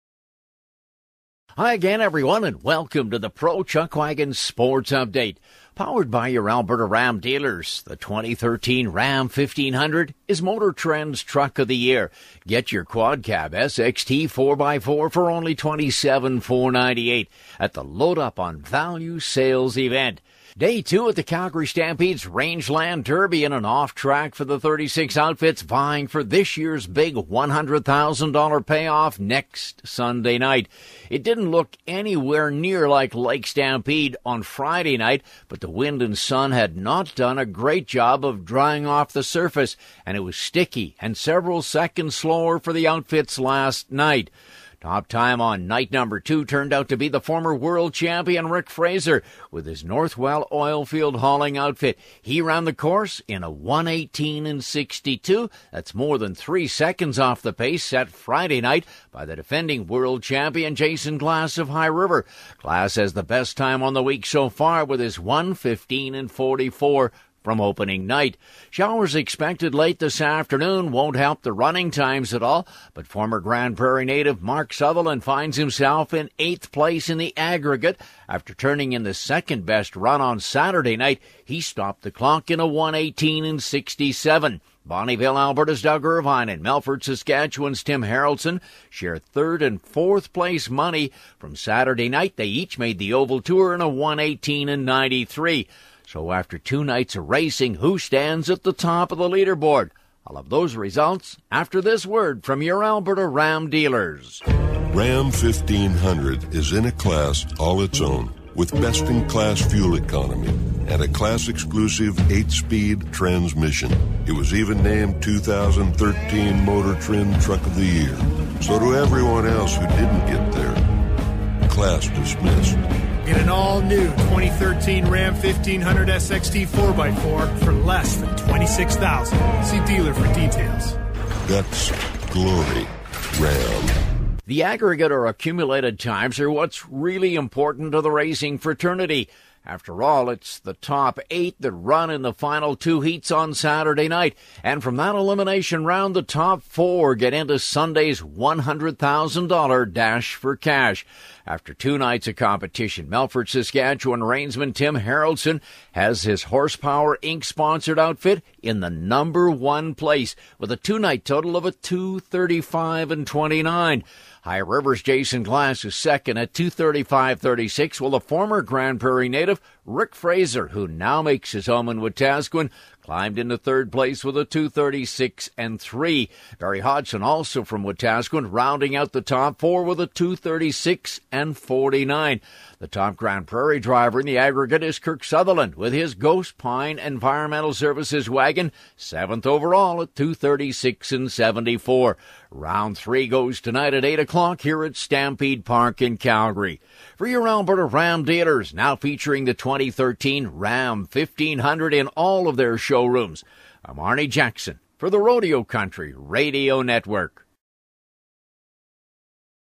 Sticky Surface to Day 2 of 2013 Calgary Stampede
Rodeo Report for Day 2 (July 6) of the 2013 Calgary Stampede by Arnie Jackson.

Pictures by Arnie Jackson.